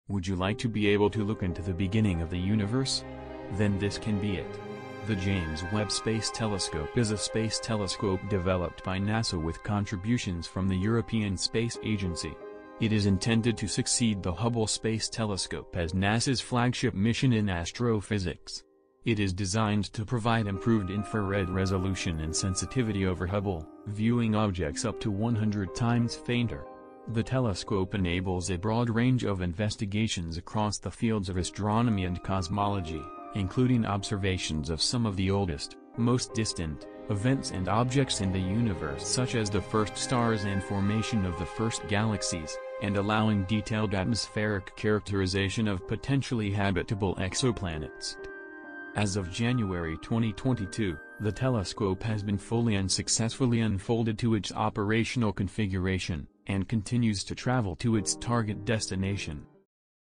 Space Facts: What will we find in space with this?
Would you like to be able to look into the beginning of the universe? Then this can be it!

The James Webb Space Telescope is a space telescope developed by NASA with contributions from the European Space Agency.

It is intended to succeed the Hubble Space Telescope as NASA's flagship mission in astrophysics.

It is designed to provide improved infrared resolution and sensitivity over Hubble, viewing objects up to 100 times fainter.

The telescope enables a broad range of investigations across the fields of astronomy and cosmology, including observations of some of the oldest, most distant, events and objects in the Universe such as the first stars and formation of the first galaxies, and allowing detailed atmospheric characterization of potentially habitable exoplanets.